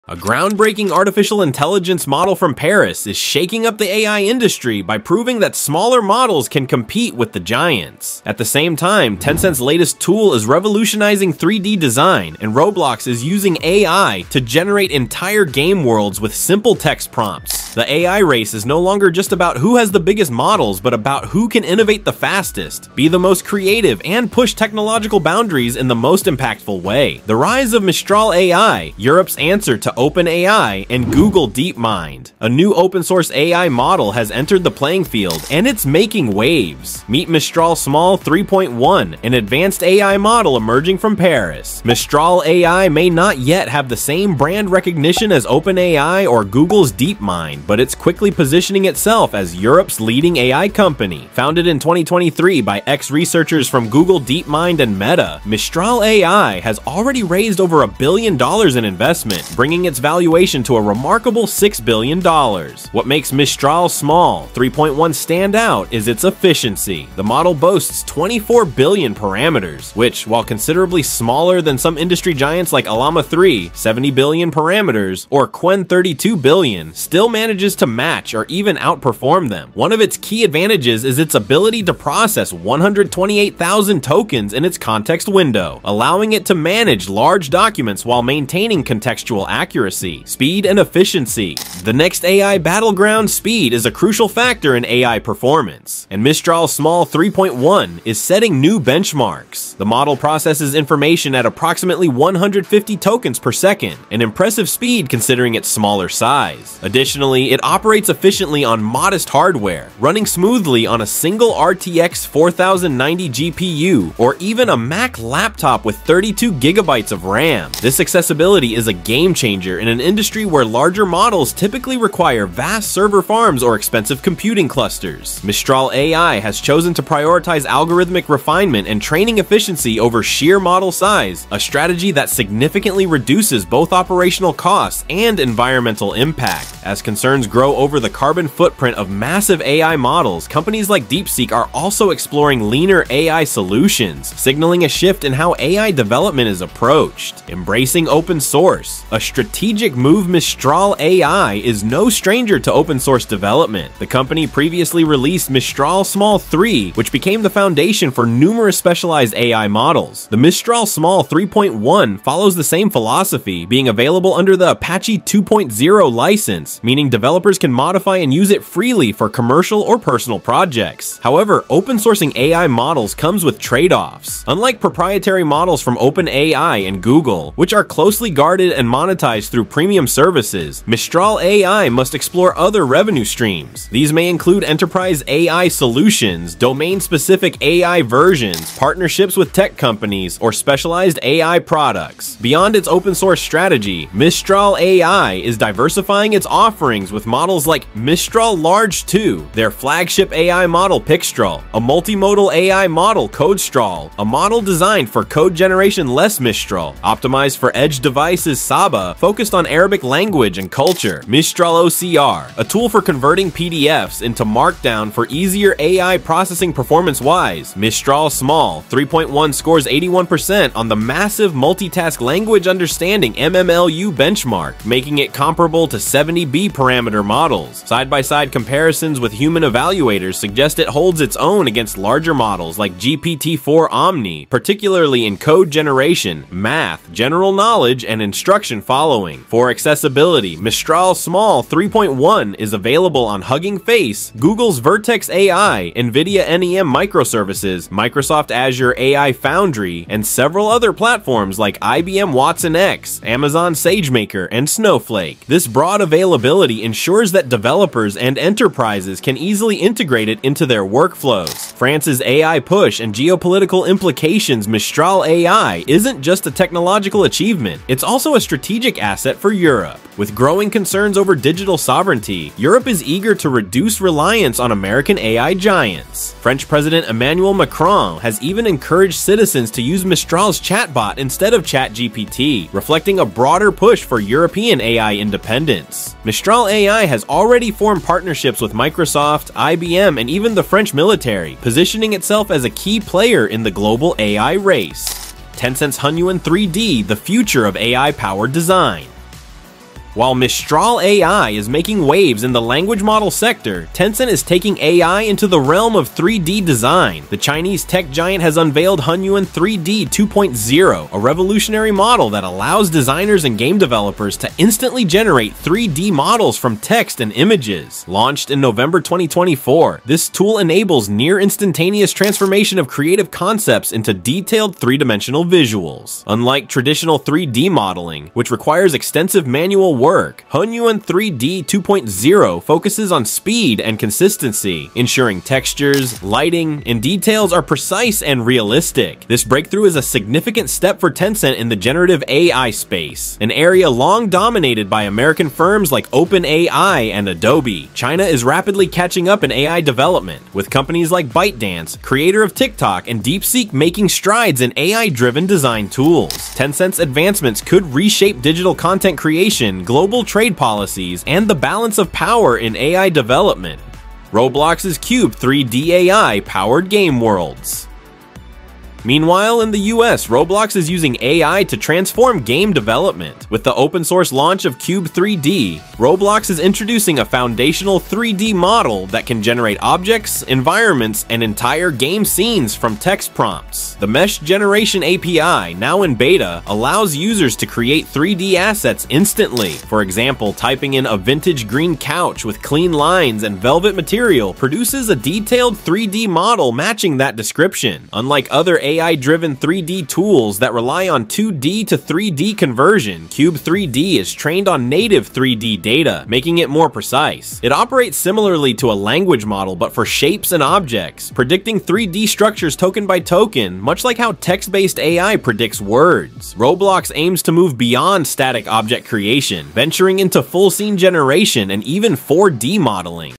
New AI Model DESTROYS GPT4o While Running on a Laptop – Mistral Small 3.1
A groundbreaking new AI model is here — and it’s crushing GPT4o with fewer resources.

Meet Mistral Small 3.1, the ultra-efficient AI developed by Paris-based Mistral AI. With 24B parameters, 128K context window, and blazing speed, it runs smoothly on just a laptop or a single RTX GPU — yet outperforms some of the biggest models out there.

And it doesn’t stop there. Tencent’s Hunyuan3D-2.0 is turning simple text into stunning 3D objects, while Roblox’s Cube 3D builds entire gaming worlds using basic prompts. These tools are reshaping AI, design, and content creation as we know it.

Whether you’re a dev, gamer, or AI enthusiast — this video is packed with the next-gen tech that will blow your mind.

🔑 KEY TAKEAWAYS:
How Mistral 3.1 beats GPT4o in key benchmarks with minimal hardware
How Hunyuan3D-2.0 is revolutionizing 3D design with pure AI
Why Cube 3D is redefining asset generation and gaming worlds
🚀 WHY IT MATTERS:
These innovations prove that the future of AI is faster, smarter, and more accessible than ever — even without high-end setups.